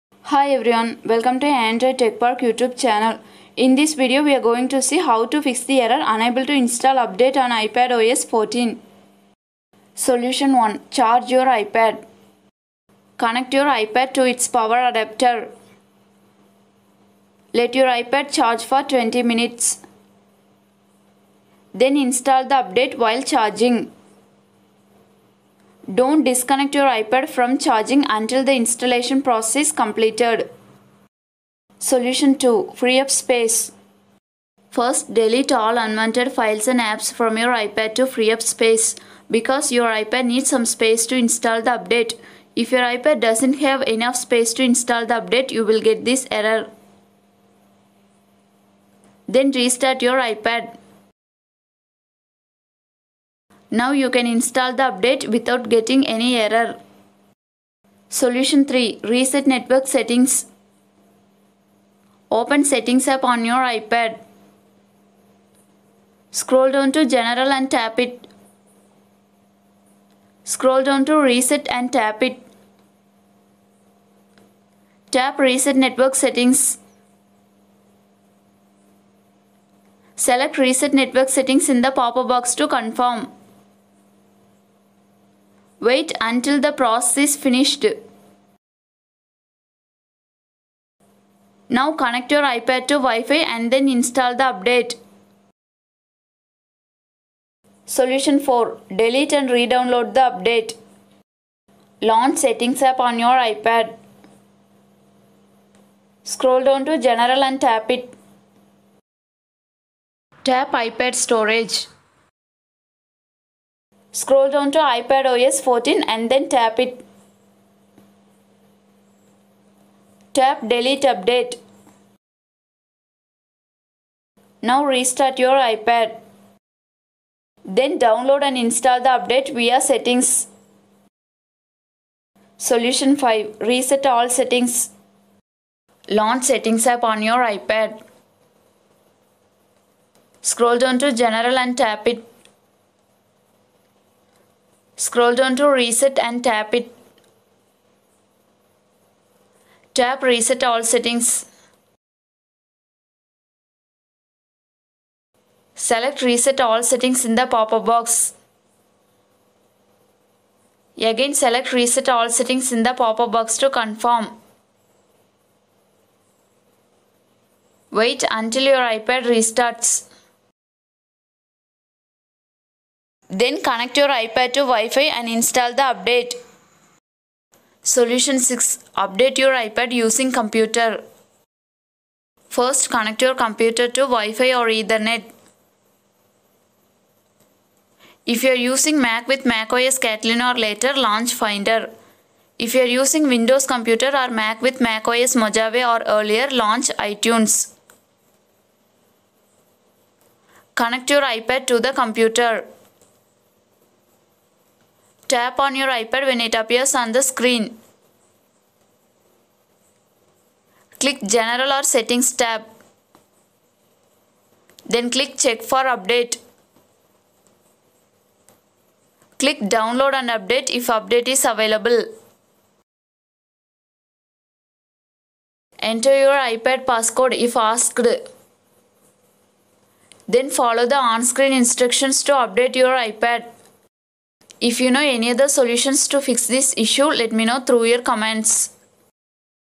Unable to Install Update iPadOS 14.5 - An Error Occurred Installing iPadOS 14.5 (Fixed)
iPad unable to install update 2020, unable to install update on ipad pro, mini, air.